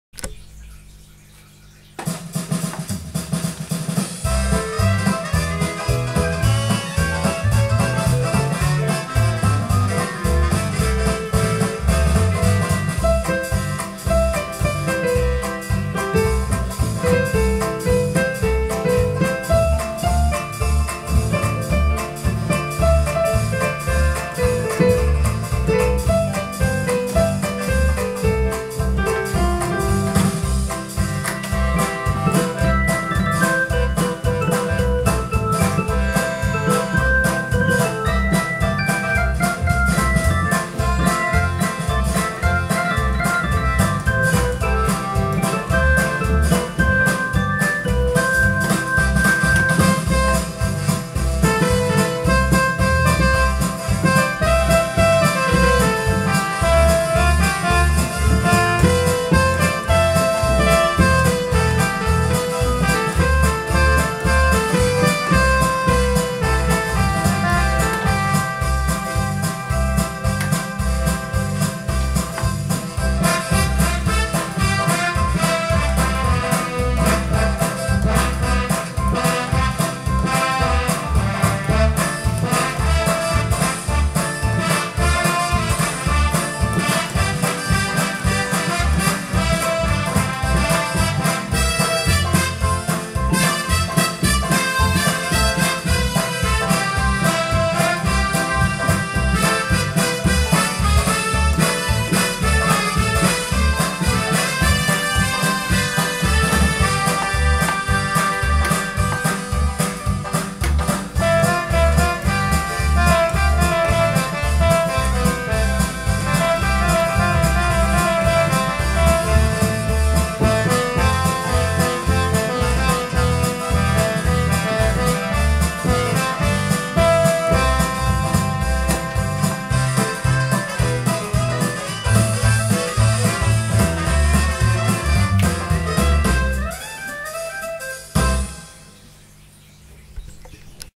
dixie halloween
j'utilise le ketron sd40 avec le yamaha clavinova je suis auteur de ma musique